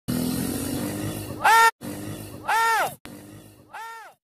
Suzuki Z90 dumps it!! Quad fail!
Tough kid! Nation is a natural rider! So much more ATV videos coming! More in my  channel now! Got right back on that lil Suzuki! Him and his brother River are good kids I just wanted them to experience riding four wheelers and get off the Xbox once in awhile! Goal attained!!